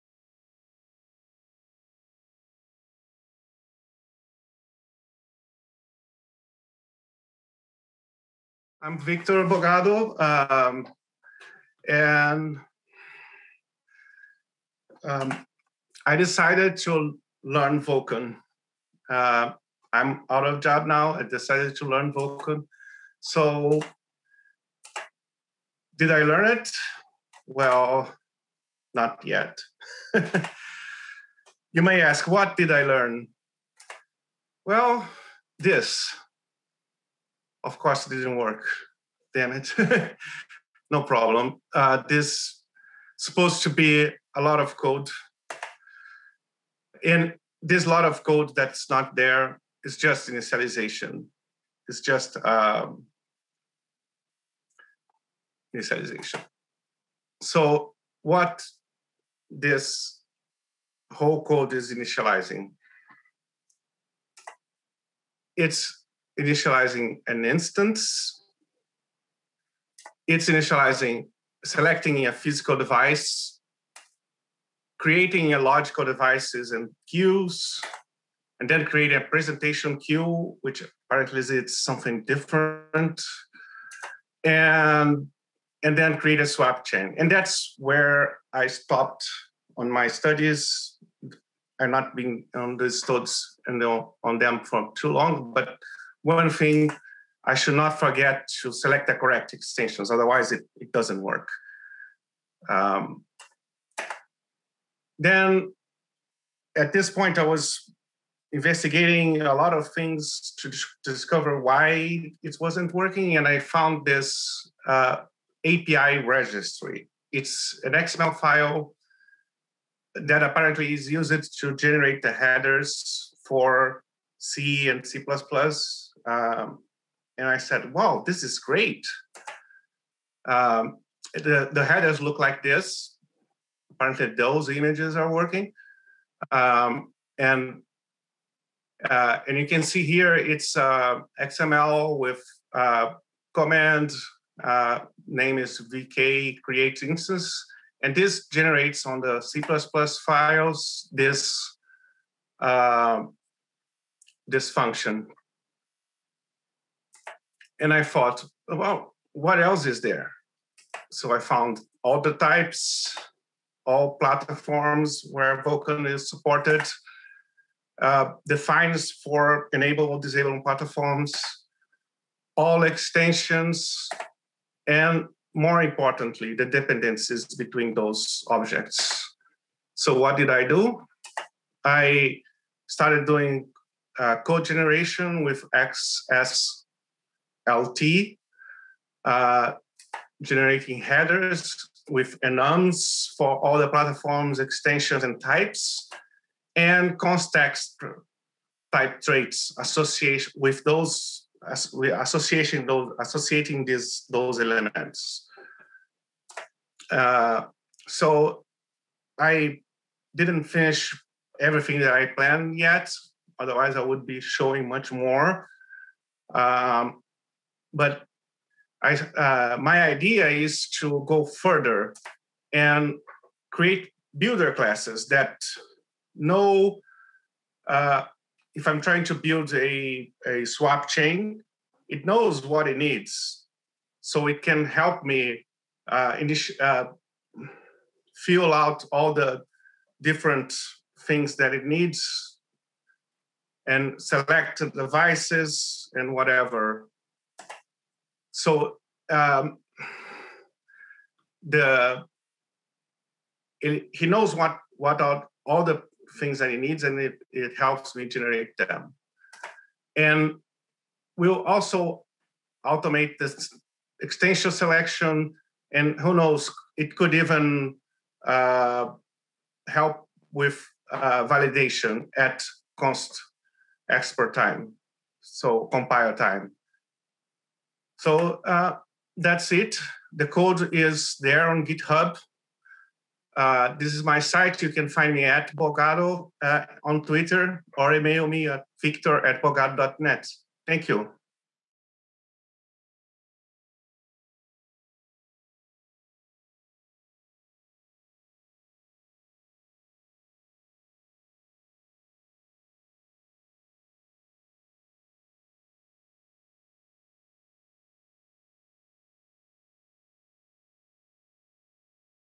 Lightning Talk: Simplify Vulkan APIs with Code Generation - Victor Bogado da Silva Lins - CppCon 21
Lightning Talk: Simplify Vulkan APIs with Code Generation

The Vulkan C++ "hello world" is huge, but the whole API is specified using an XML file. Those specs can be used on code generation to generate constexpr code that can simplify many of the tasks done on the initialization process.
Victor Bogado da Silva Lins

I've been involved with C++ since the time when c++11 was known as C++0x. I'm a learner by nature and C++, as a language where you never stop learning, is my passion.